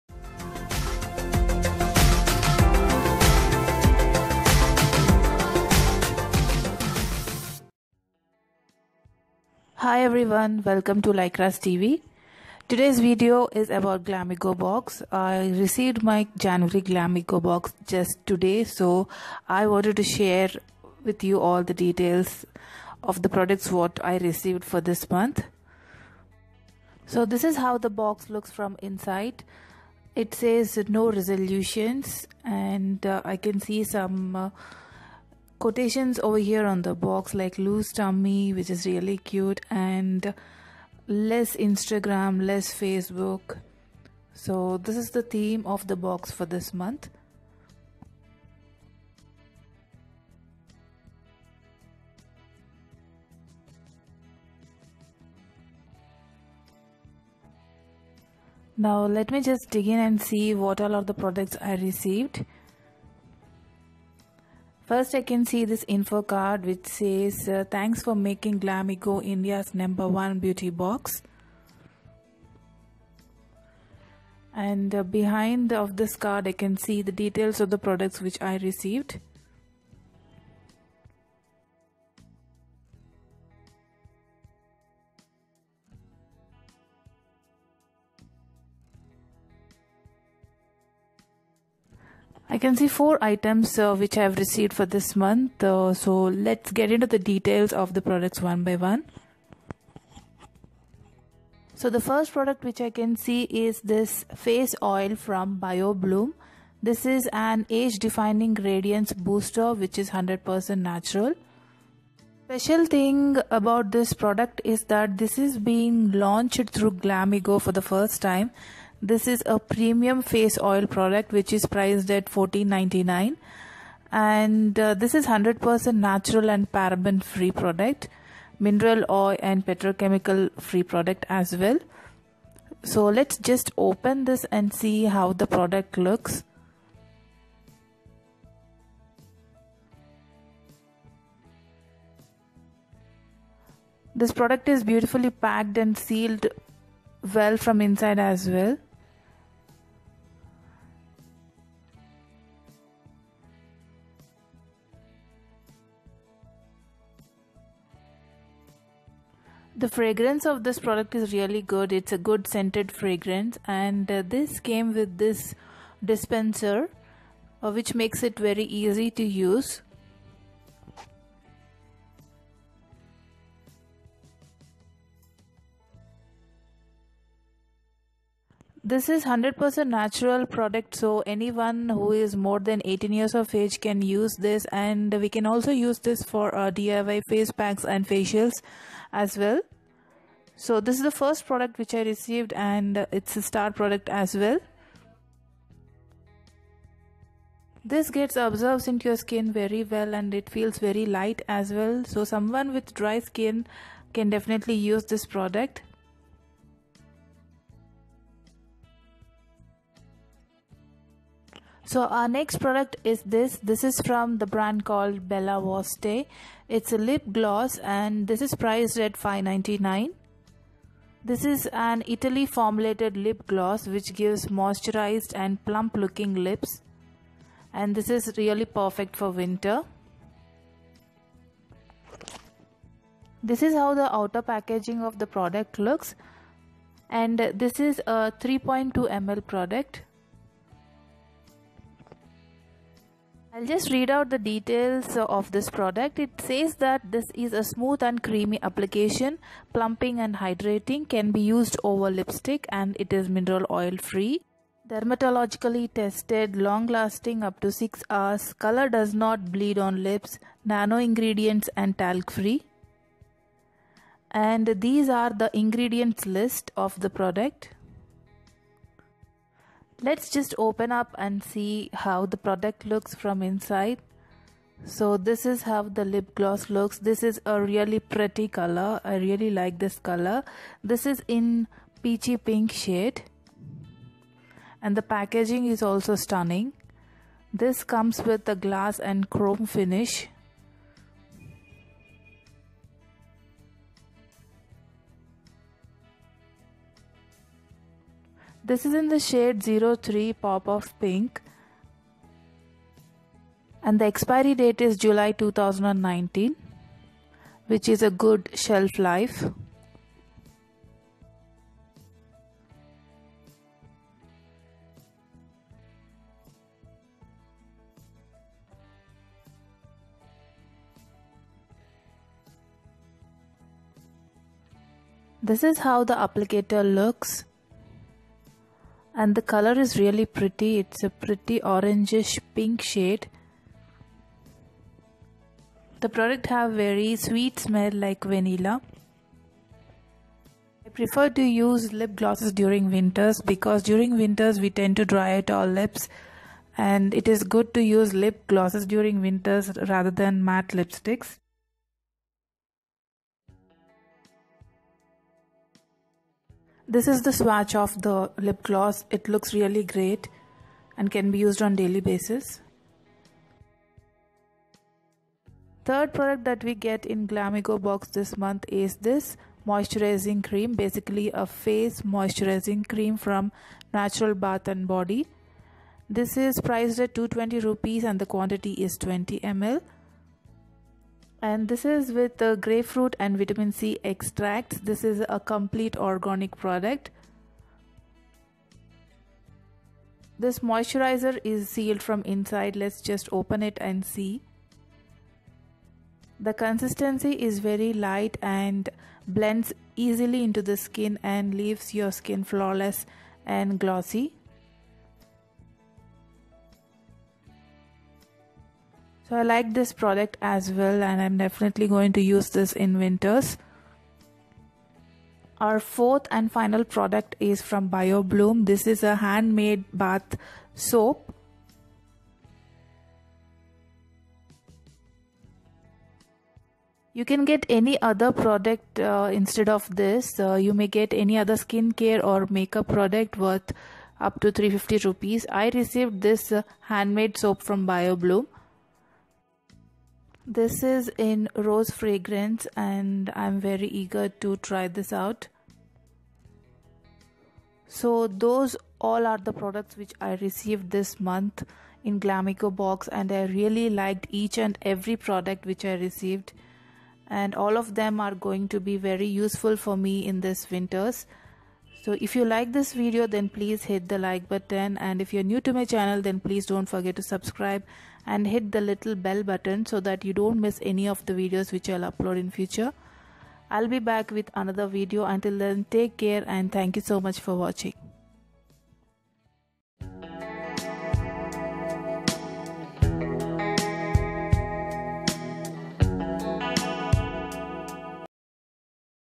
Glamego Box January 2018- India's most affordable beauty subscription box
Hi Everyone,

Today I am sharing you the unboxing video of Glamego box for January 2018

This month's Glamego box contains products worth more than INR 2200

Pricing and subscription plan details

1 month subscription priced at INR 399
3 month subscription priced at INR 329
6 month subscriprion priced at INR 299

Review made on the products are based on my personal experience and I am not saying that it will be same for all

LaiKRa'S TV